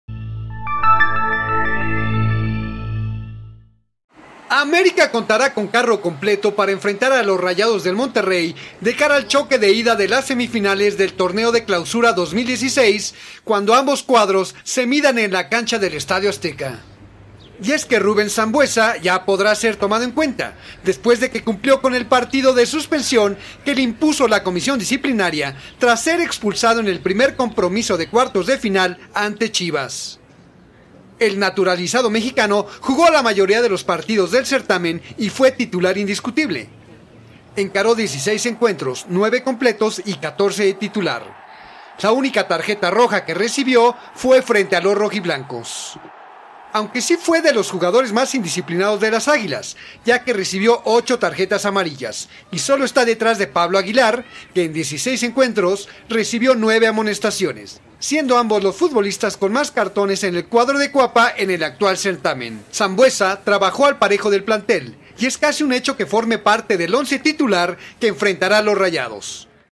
América, con carro completo para enfrentar a Monterrey
CLAUSURA 2016, Ciudad de México, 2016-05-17.- América contará con carro completo para enfrentar a los Rayados del Monterrey de cara al choque de ida de las semifinales del torneo de Clausura 2016 cuando ambos cuadros se midan en la cancha del Estadio Azteca.